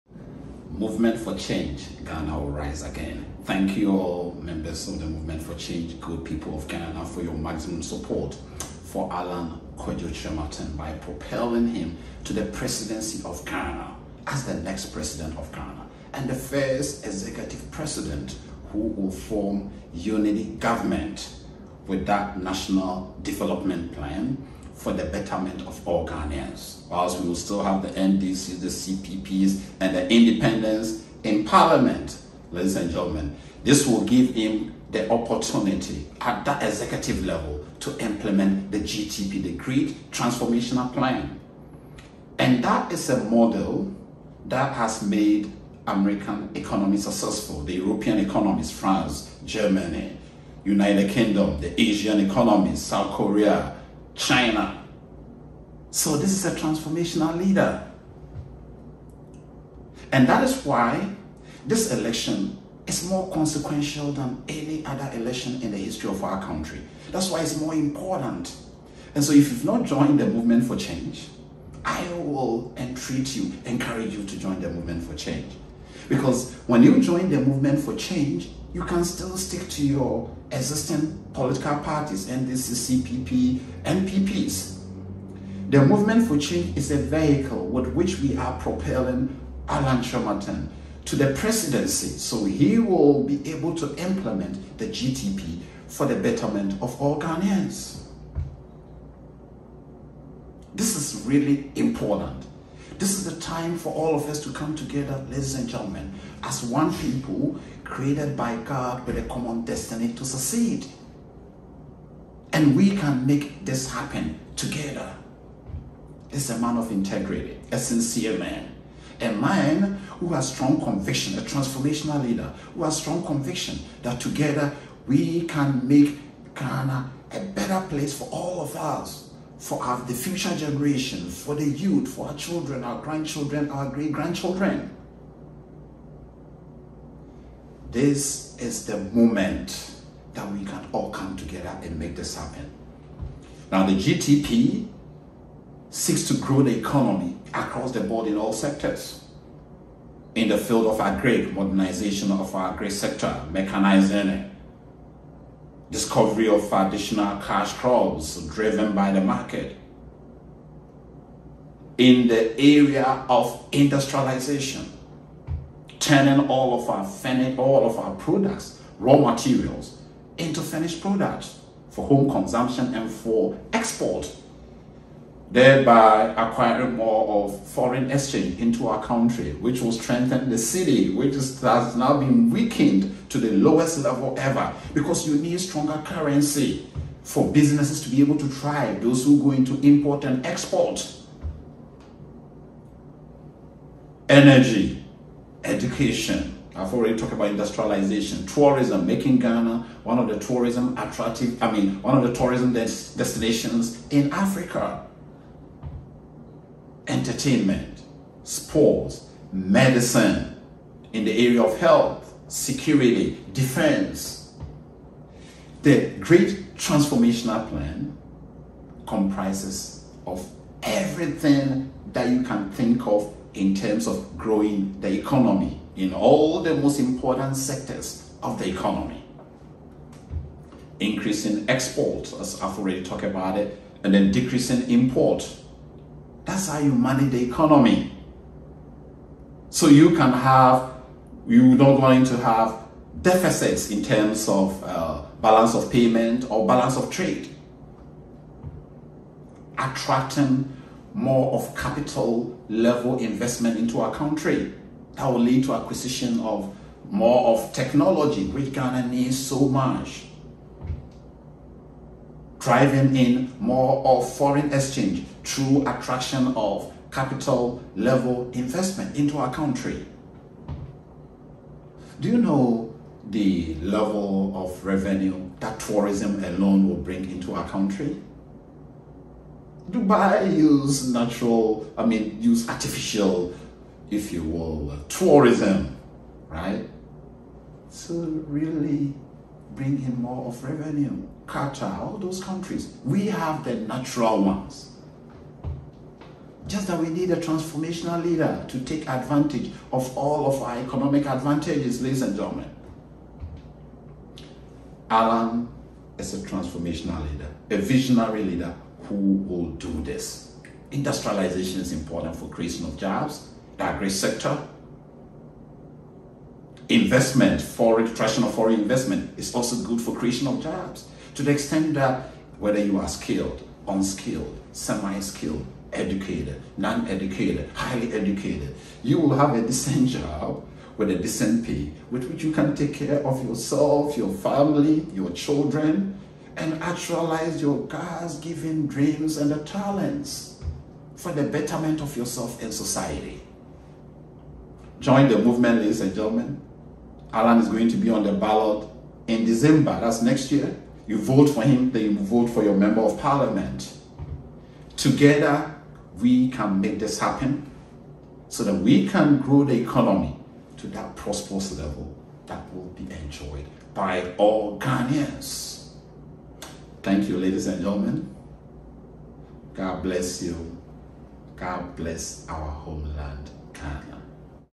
Movement for Change (Alan Kyerematen) and the GTP(Great Transformational Plan) Explained
The Movement for Change and the GTP( the Great Transformational Plan ) led by Alan Kyerematen, the Presidential Candidate of Ghana have been explained in detail in this video by Francis Asante (Finance Lecturer in USA)